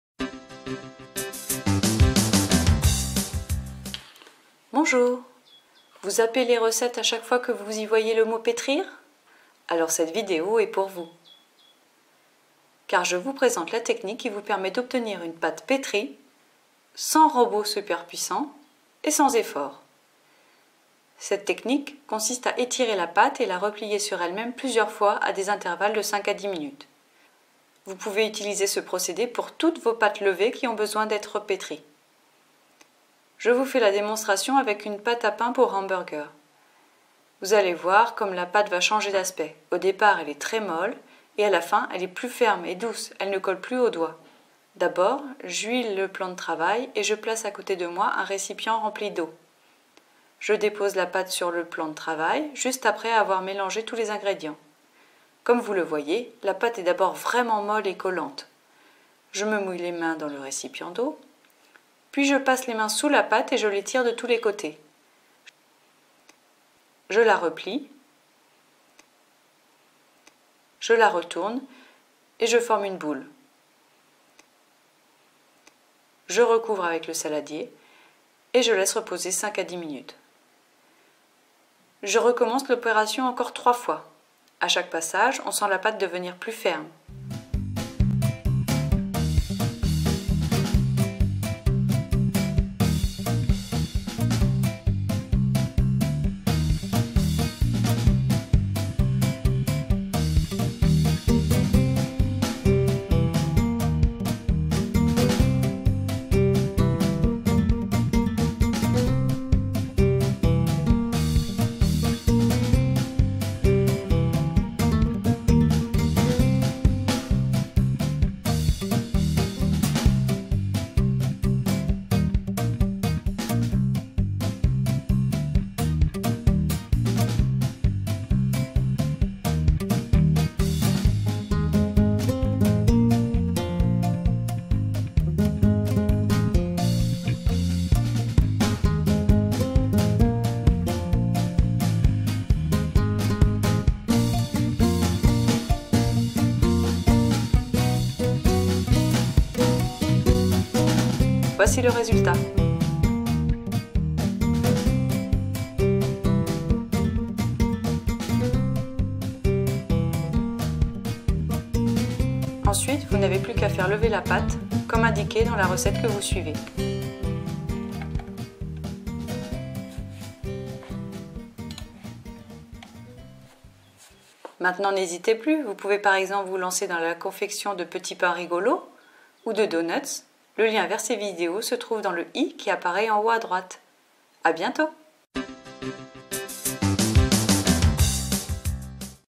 PAIN: LA technique qui remplace le pétrissage
Vous zappez les recettes à chaque fois que vous y voyez le mot pétrir ? Alors cette vidéo s’adresse à vous! Car je vous présente la technique qui vous permet d’obtenir une pâte pétrie sans robot super puissant et sans effort.

D'après la technique de Peter Reinhart

Artiste : __url_artiste__